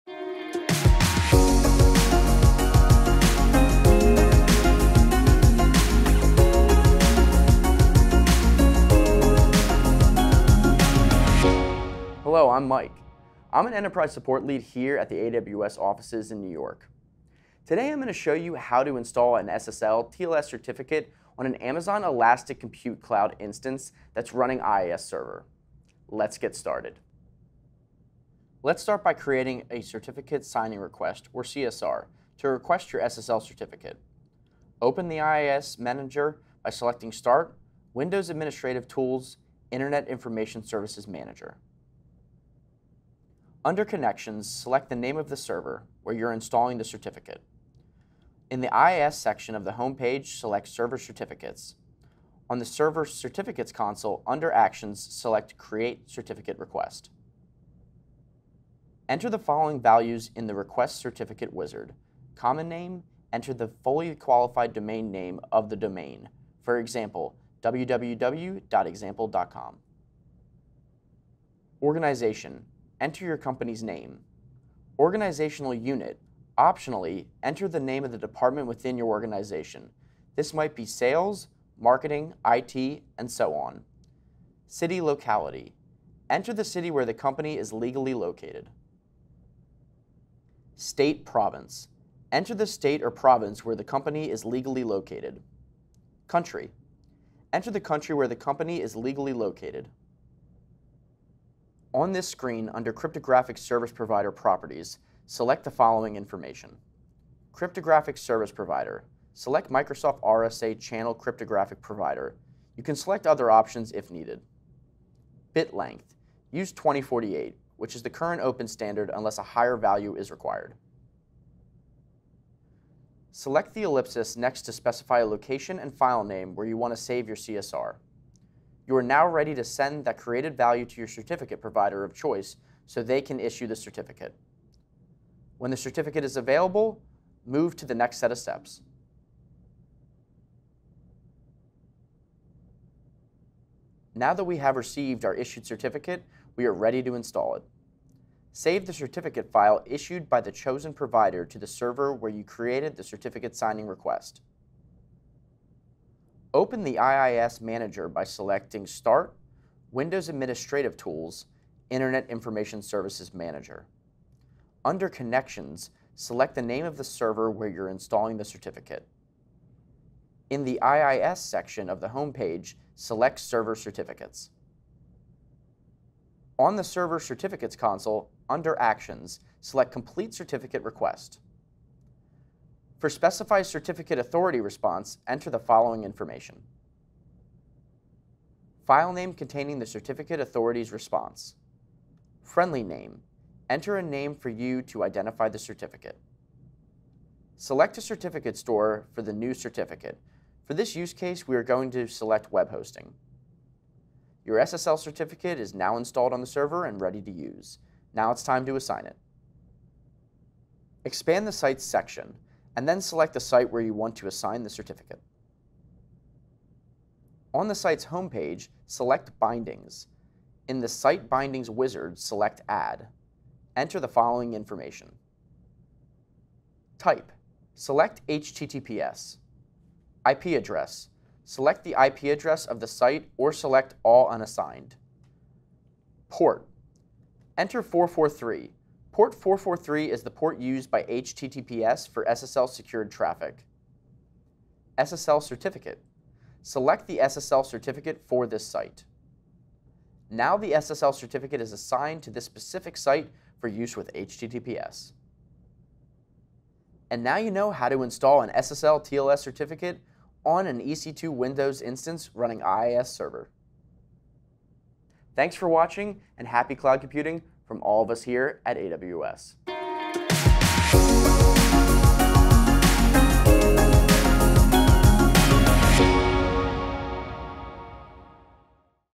How do I install an SSL/TLS certificate on my EC2 Windows instance running IIS server?
Skip directly to the demo: 0:27

For more details see the Knowledge Center article with this video

Mike shows you how to install an SSL/TLS certificate on your EC2 Windows instance running IIS server.

0:00 Introduction
0:27 Create a Certificate Signing Request
2:22 Install your certificate
3:30 Assign your certificate
4:23 Closing